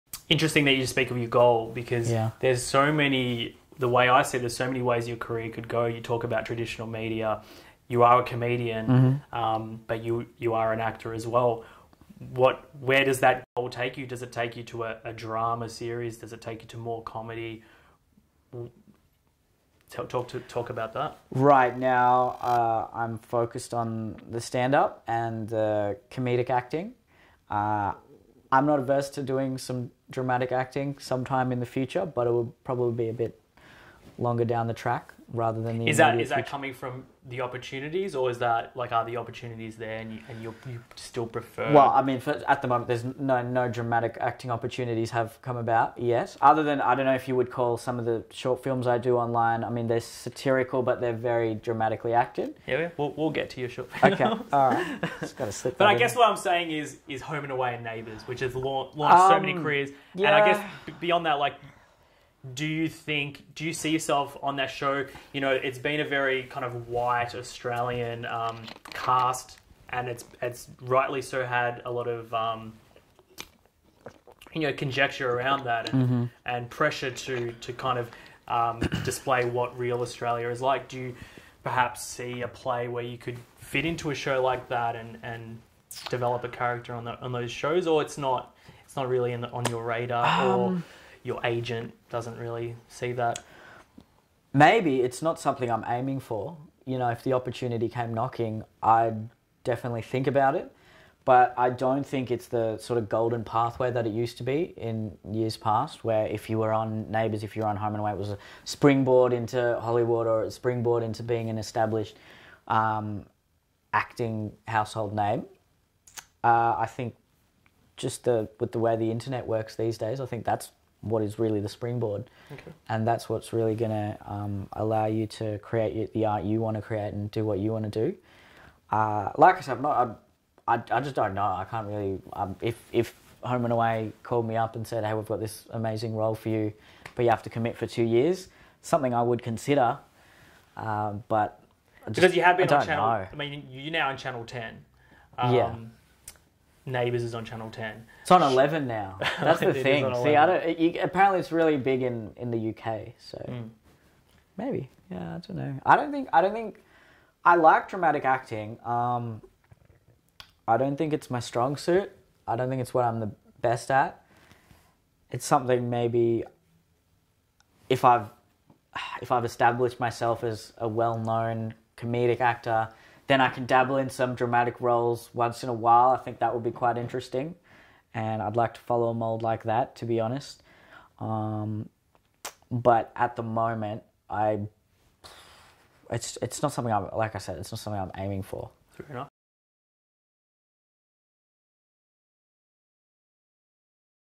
Neel Kolhatkar on Home & Away or Neighbours? | Neel Kolhatkar Interview | Harry Ax Podcast
Part 2 of my interview with Neel Kolhatkar